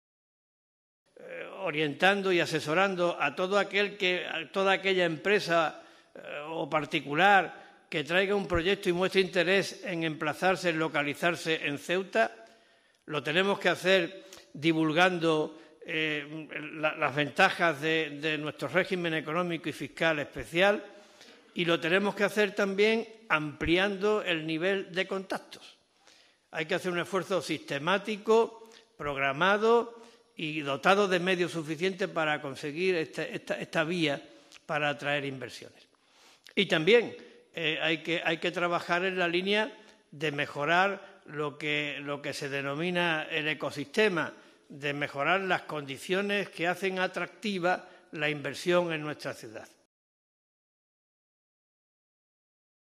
Vivas considera un reto fundamental conseguir atraer inversión desde el exterior
El presidente de la Ciudad también considera un reto fundamental de este año, conseguir atraer inversión desde el exterior. Vivas asegura que Ceuta tiene condiciones que la hace atractiva para los inversores. Y en la dirección de este objetivo, hay que hacer un esfuerzo de asesoramiento y divulgación de las ventajas fiscales, así como en ampliar el nivel de contactos.